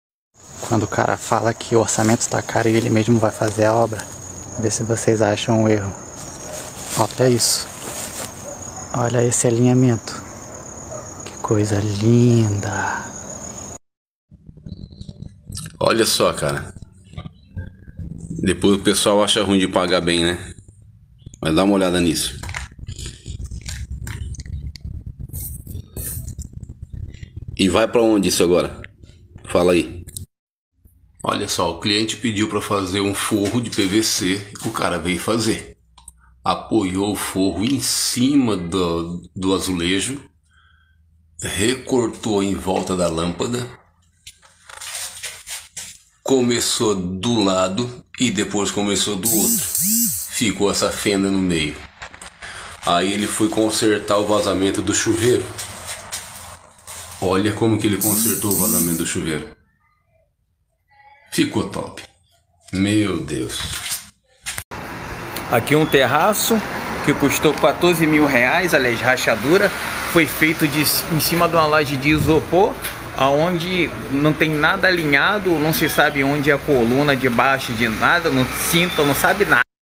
Lembrete do dia: O barato sai caro
Este vídeo é uma compilação repleta de situações engraçadas e inusitadas que nos lembram da velha máxima: economizar nem sempre é a melhor opção. Das escolhas mais econômicas que resultam em caos até as soluções improvisadas que surpreendem, cada cena é um lembrete humorístico de que, muitas vezes, o barato pode custar caro. Divirta-se com estas lições cômicas e aproveite para refletir sobre a importância de fazer escolhas conscientes. Assista agora e deixe o riso iluminar seu dia!